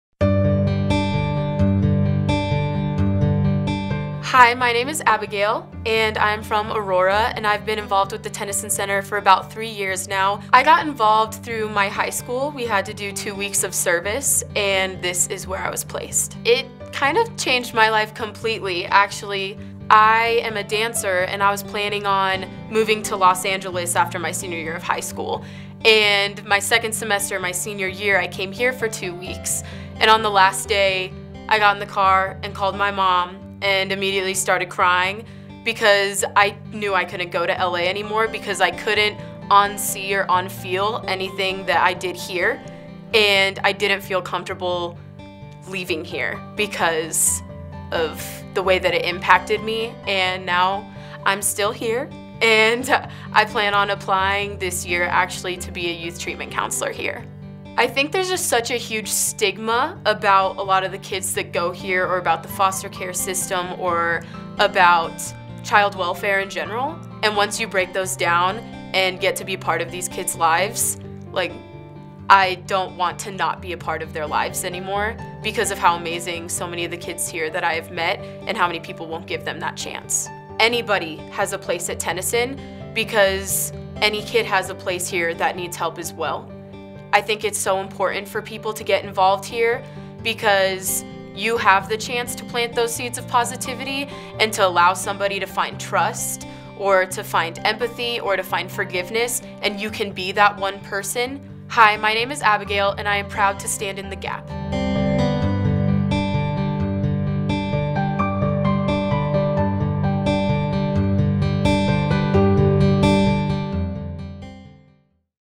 Abagail
Abagail grew up wanting to become a dancer in LA. Two weeks into volunteering at Tennyson her plans changed. "I knew I couldn't go to LA anymore," she said. "I had to keep helping these kids."